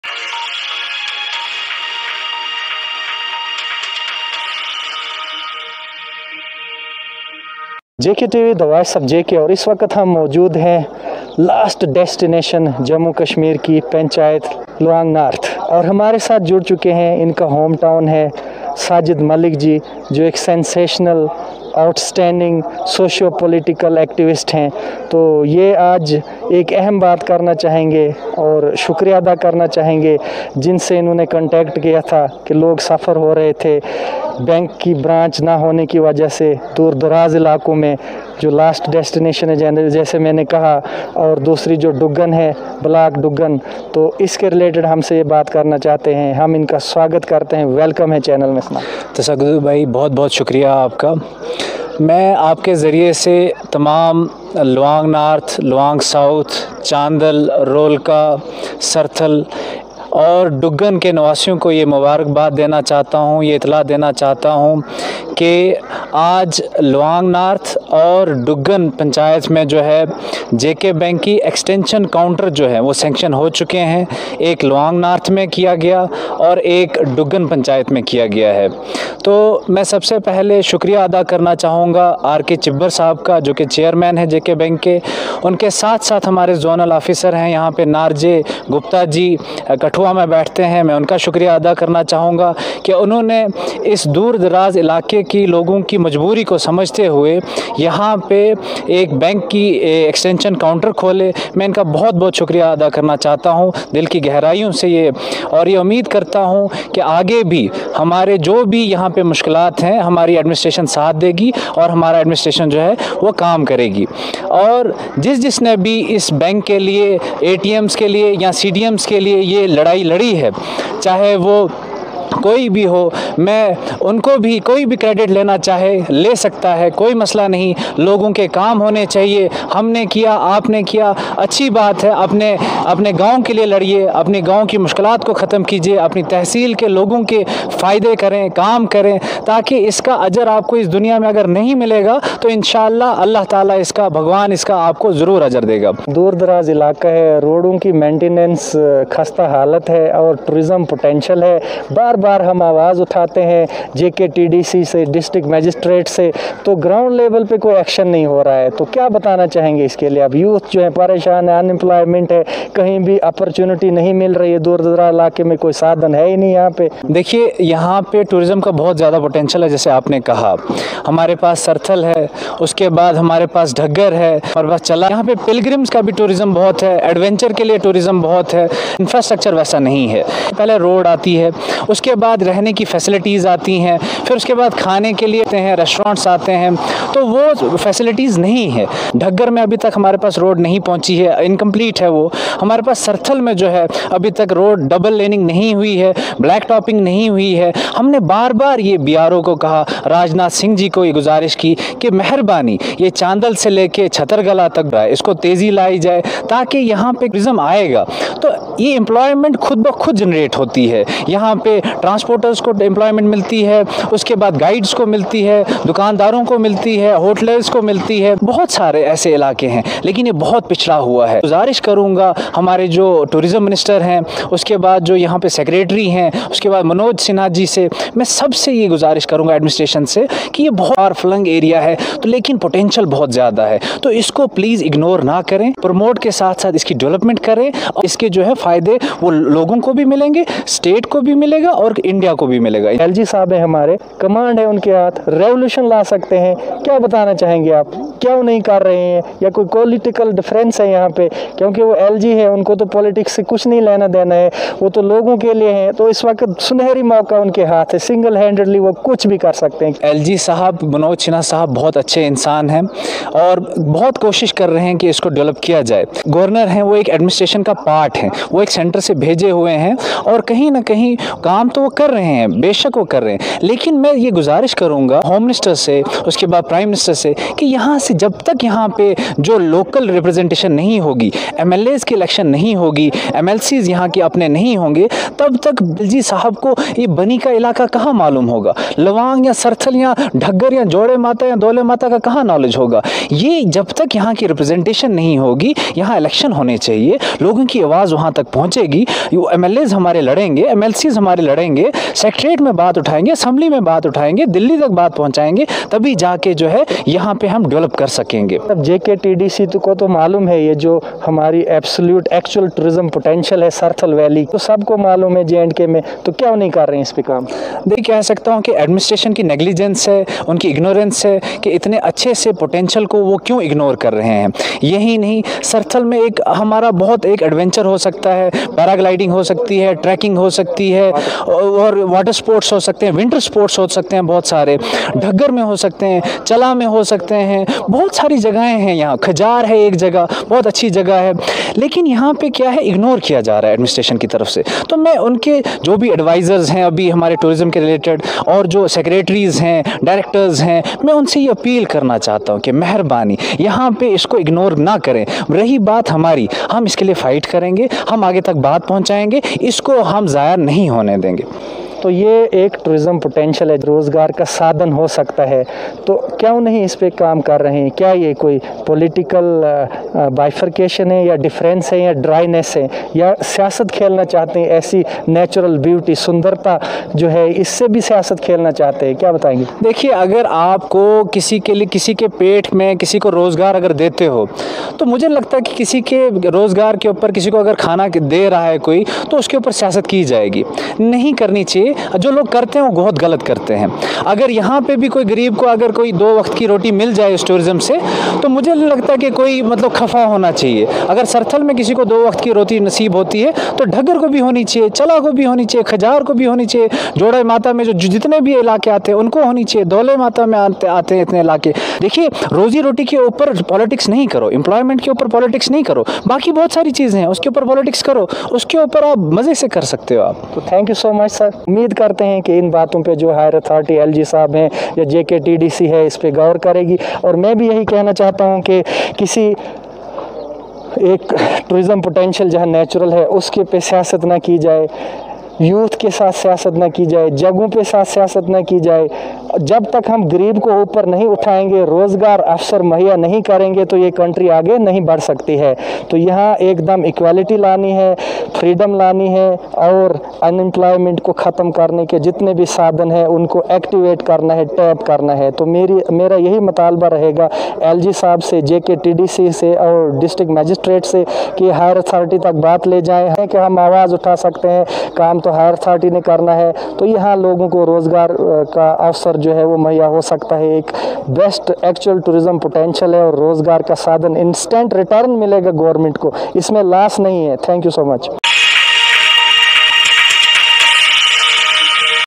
Mr Sajid Malik activist thanked Jkbank for granting two extension counters in far-flung Bani J&K!
A sensational socio political activist Mr Sajid Malik thanked Jkbank chair man and zonal Officer Kathua for granting instant extension counters in far-flung Tehsil Bani District kathua Jammu and Kashmir to facilitate the people and end sufferings being farthest inaccessible place to outreach. Apart from that Mr Sajid urges LG sahab Jammu and Kashmir to boost Tourism and all possible developmental activities to end up unemployment and ensure bright future ahead to make nation and country strong .He further elaborated that all political parties leadership must not play regional politics and our motive should be above political influence to meet fundamental rights of the people and secure democracy in its absolute pure sense.